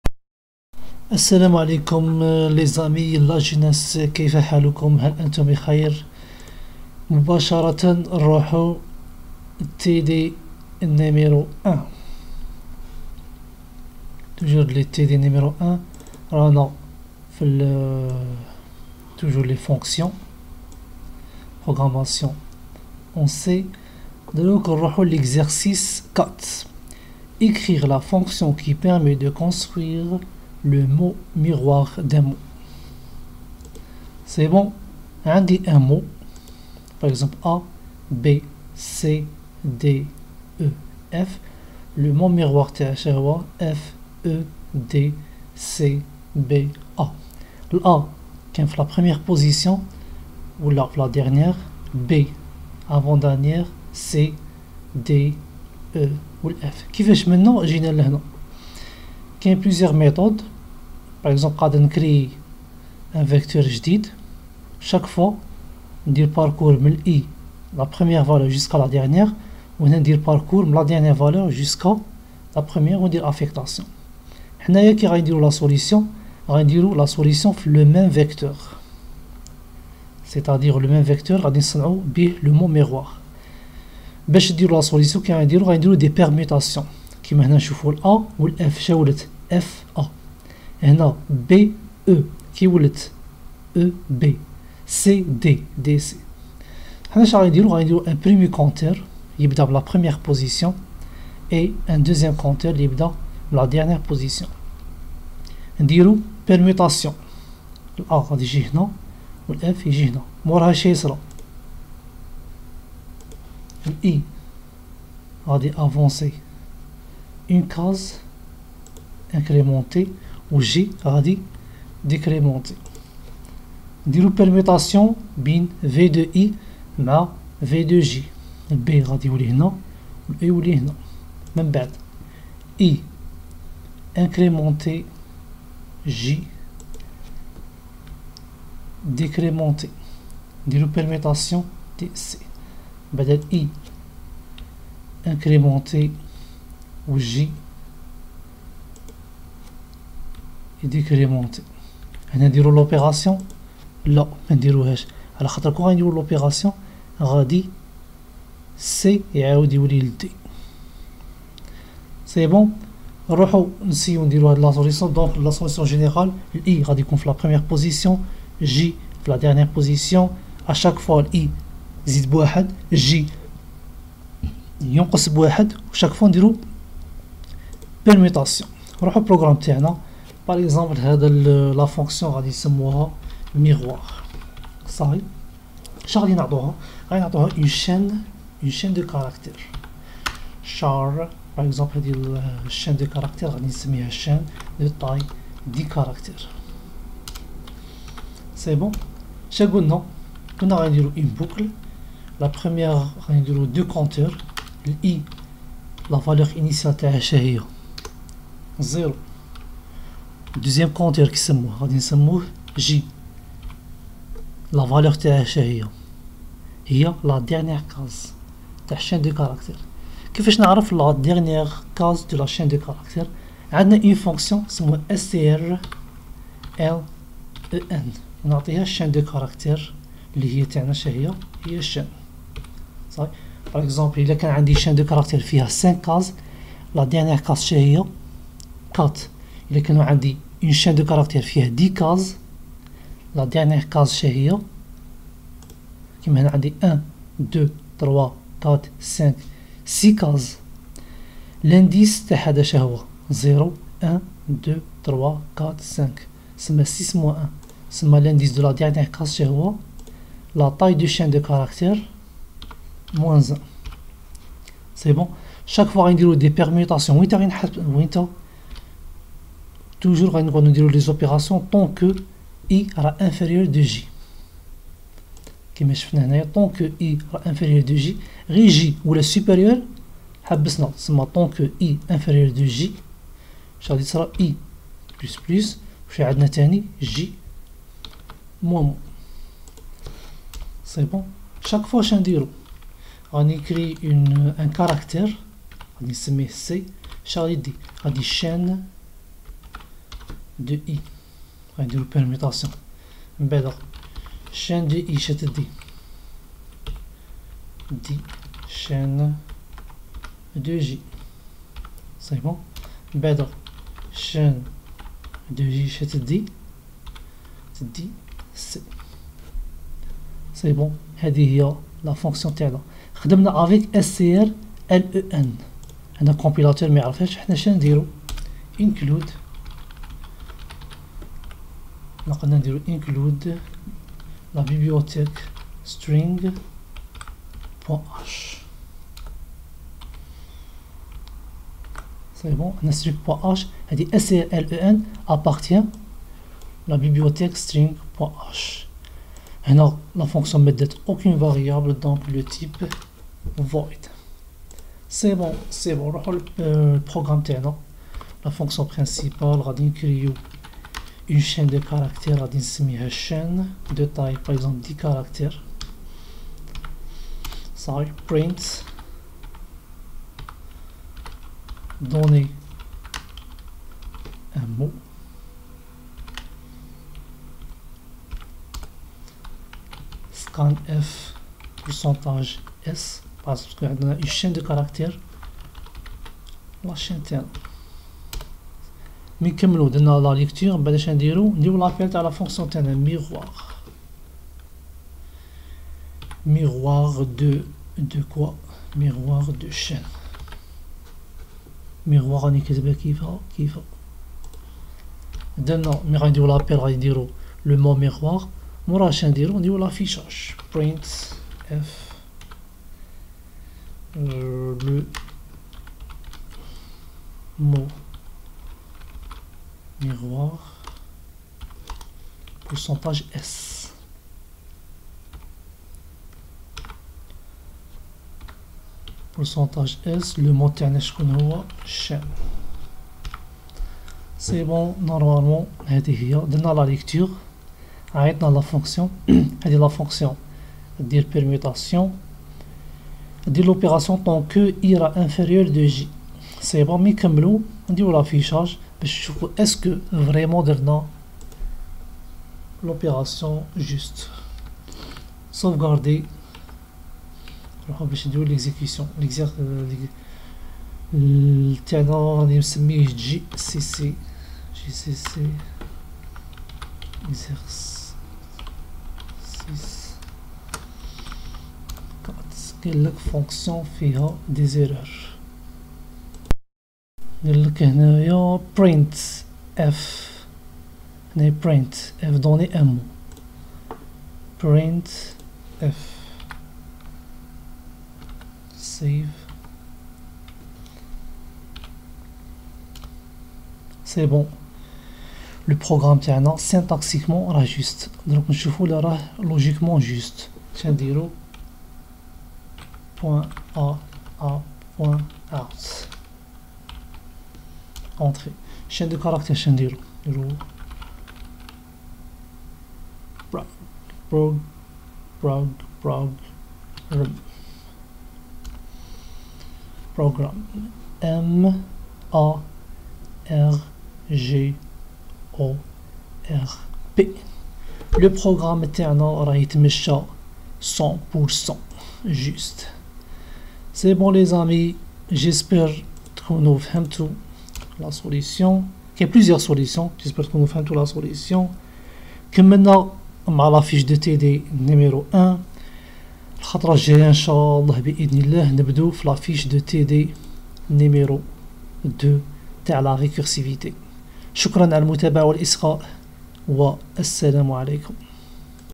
Fiches de TD ASD 2 - TD 1 - Les fonctions en C - Exercice 4
faculté des mathématiques et d'informatique
Département d'informatique

1er année licence
module : ASD 2 (Algorithmique et structures de données).

cette série de video contient toutes les solutions de tous les exercices des fiches de TD du module ASD

programmation en C
Exercice 4:Ecrire la fonction qui permet de construire le mot miroir d’un mot

les fonctions
la récursivité
les fichiers
les listes
les piles